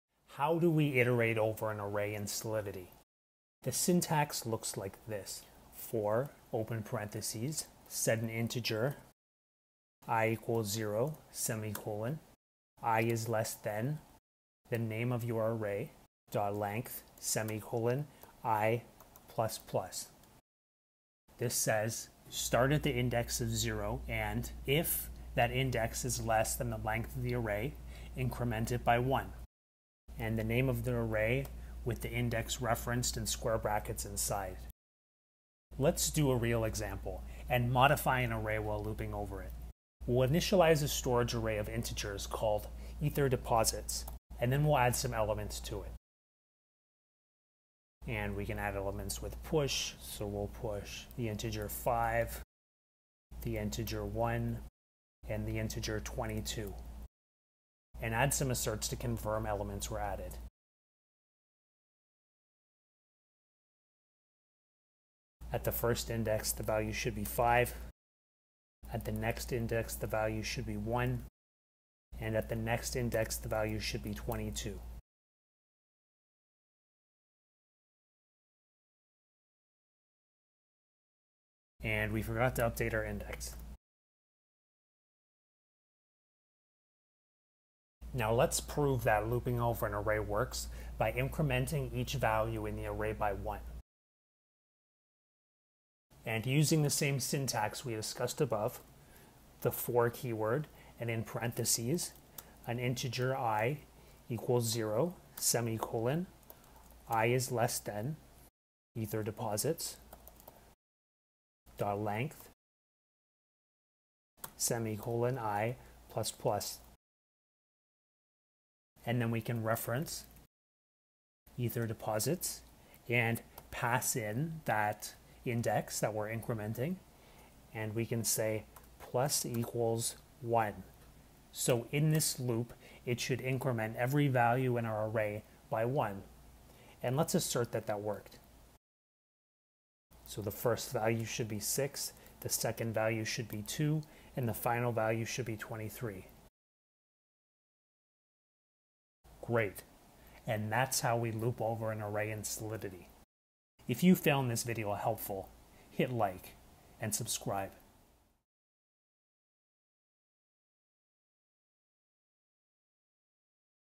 Looping on Arrays in Solidity
Let's learn to loop over an array in solidity.

0:00 - Syntax
0:39 - Define an array
0:44 - Add elements to the array
1:27 - Loop on the array
2:01 - Increment each element in the loop
2:18 - Add asserts to test the incrementing